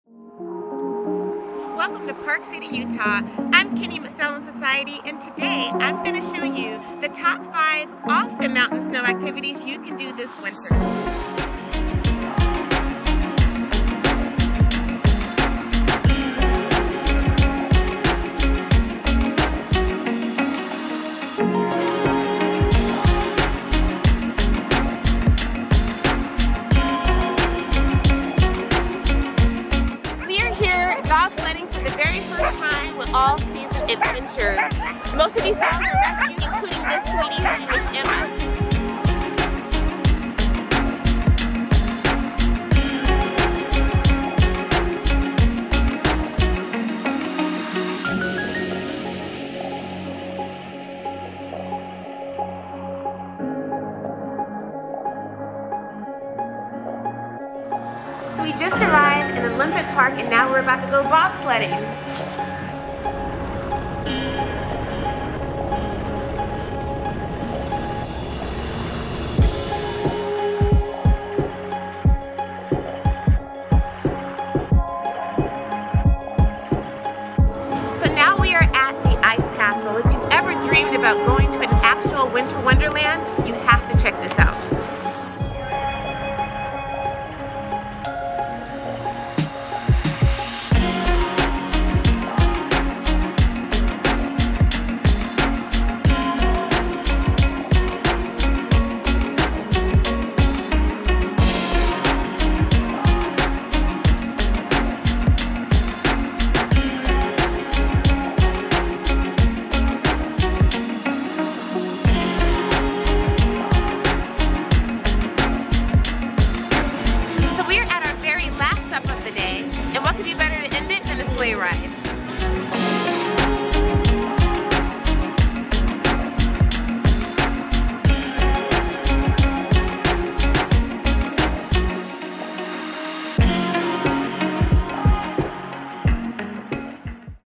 What to do in Park City Beyond the Ski Slopes
What to do in Park City Beyond the Ski Slopes? Checkout Kinya Claiborne's off-the-mountain guide to the coolest things to do in Park City, Utah besides skiing and snowboarding. See the full travel editorial feature in STYLE & SOCIETY Magazine ❄️
Destination @visitparkcity